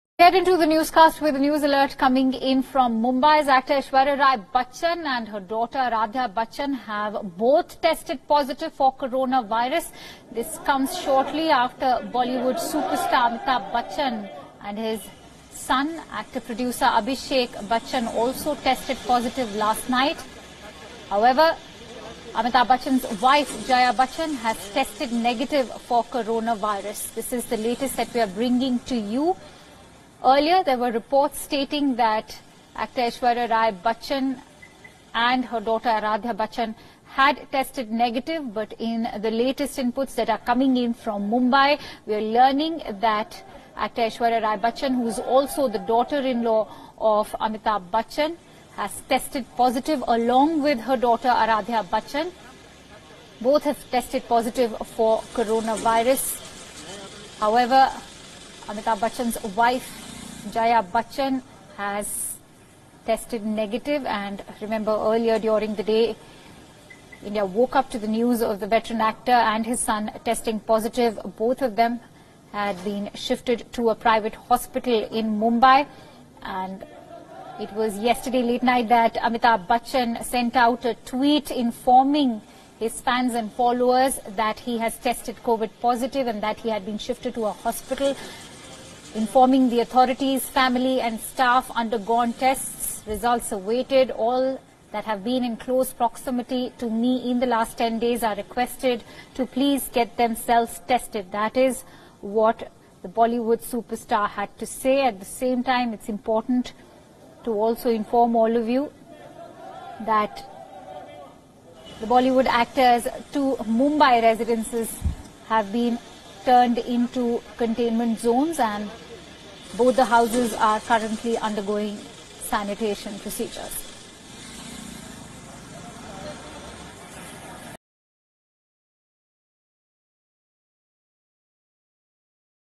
सभी को जवाब देना मुश्किल है - अमिताभ बच्चन | Big B Tests POSITIVE | Covid-19 | Nanavati Hospital
Breaking News: Everyone is difficult to answer - Amitabh Bachchan